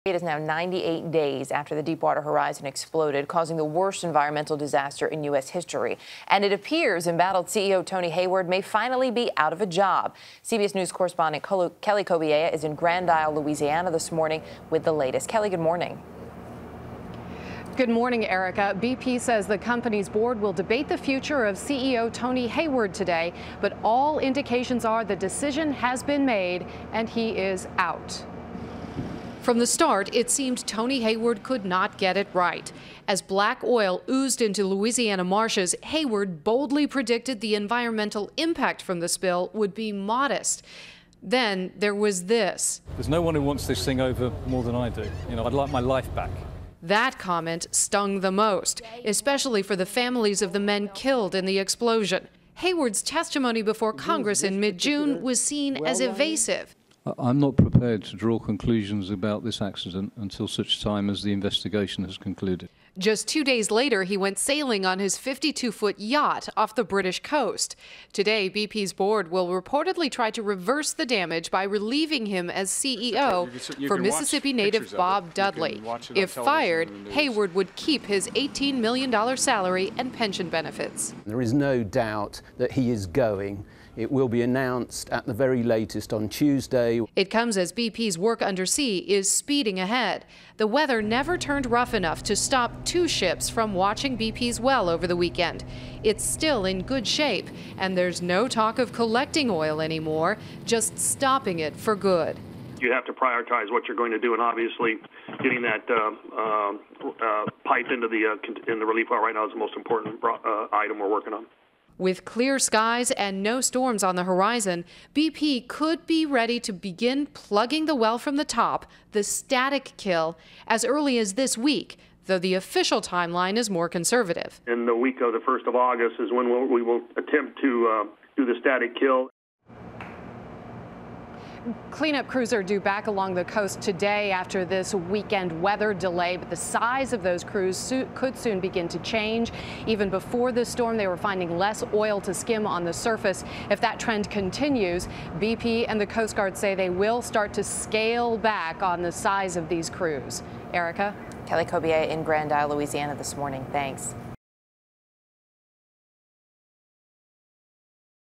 BP CEO Hayward to Step Down
Embattled BP CEO Tony Hayward is expected to step down and will be replaced by Miss. native Bob Dudley.  Kelly Cobiella reports.